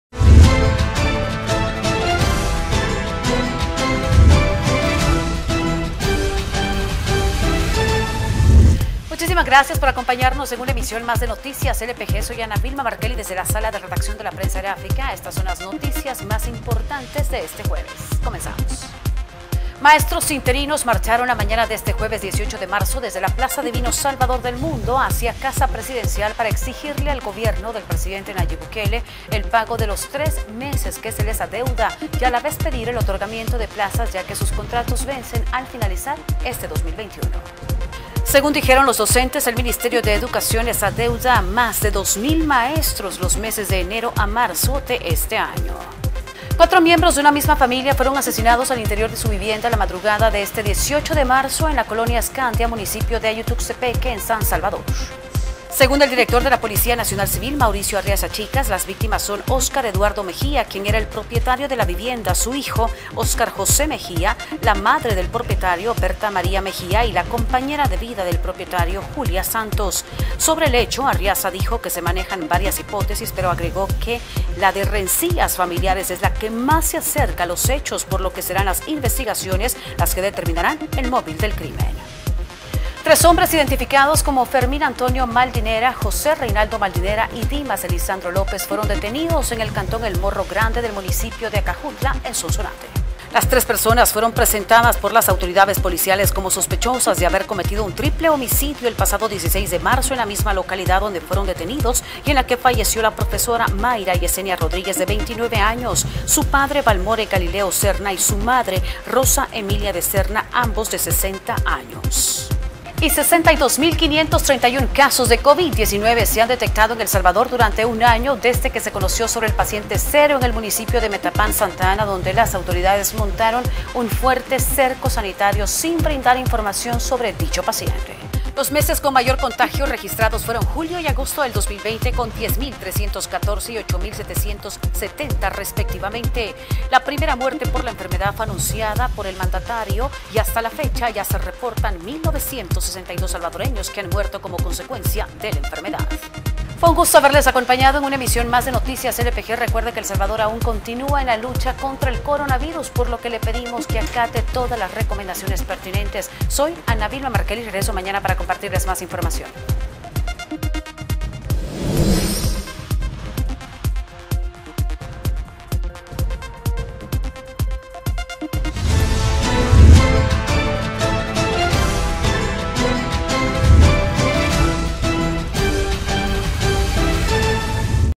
Noticiero LPG 18 de marzo: Asesinan a cuatro miembros de una familia en vivienda de Ayutuxtepeque
Cuatro miembros de una misma familia fueron asesinados al interior de su vivienda la madrugada de este 18  de marzo, en la colonia Scandia, Municipio de Ayutuxtepeque, en San Salvador.

¡Visítenos!